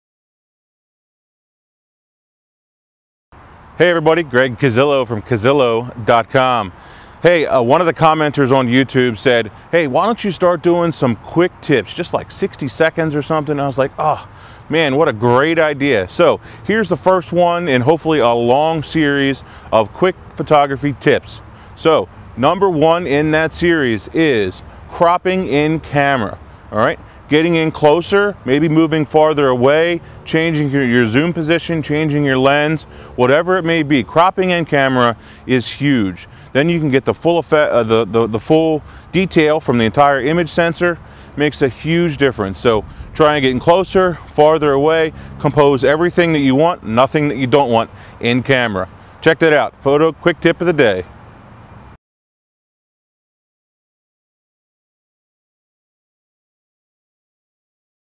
Crop in Camera - Photography Quick Tip #1
New Segment! Quick tips are a new series (thanks to a YouTube commenter!) of videos I'm doing to help improve your photography.  Each quick tip will be easy to impliment and use as well as super short.  This first in a long series is about cropping in camera for better photos.  Check it out and let me know what you think of this new series!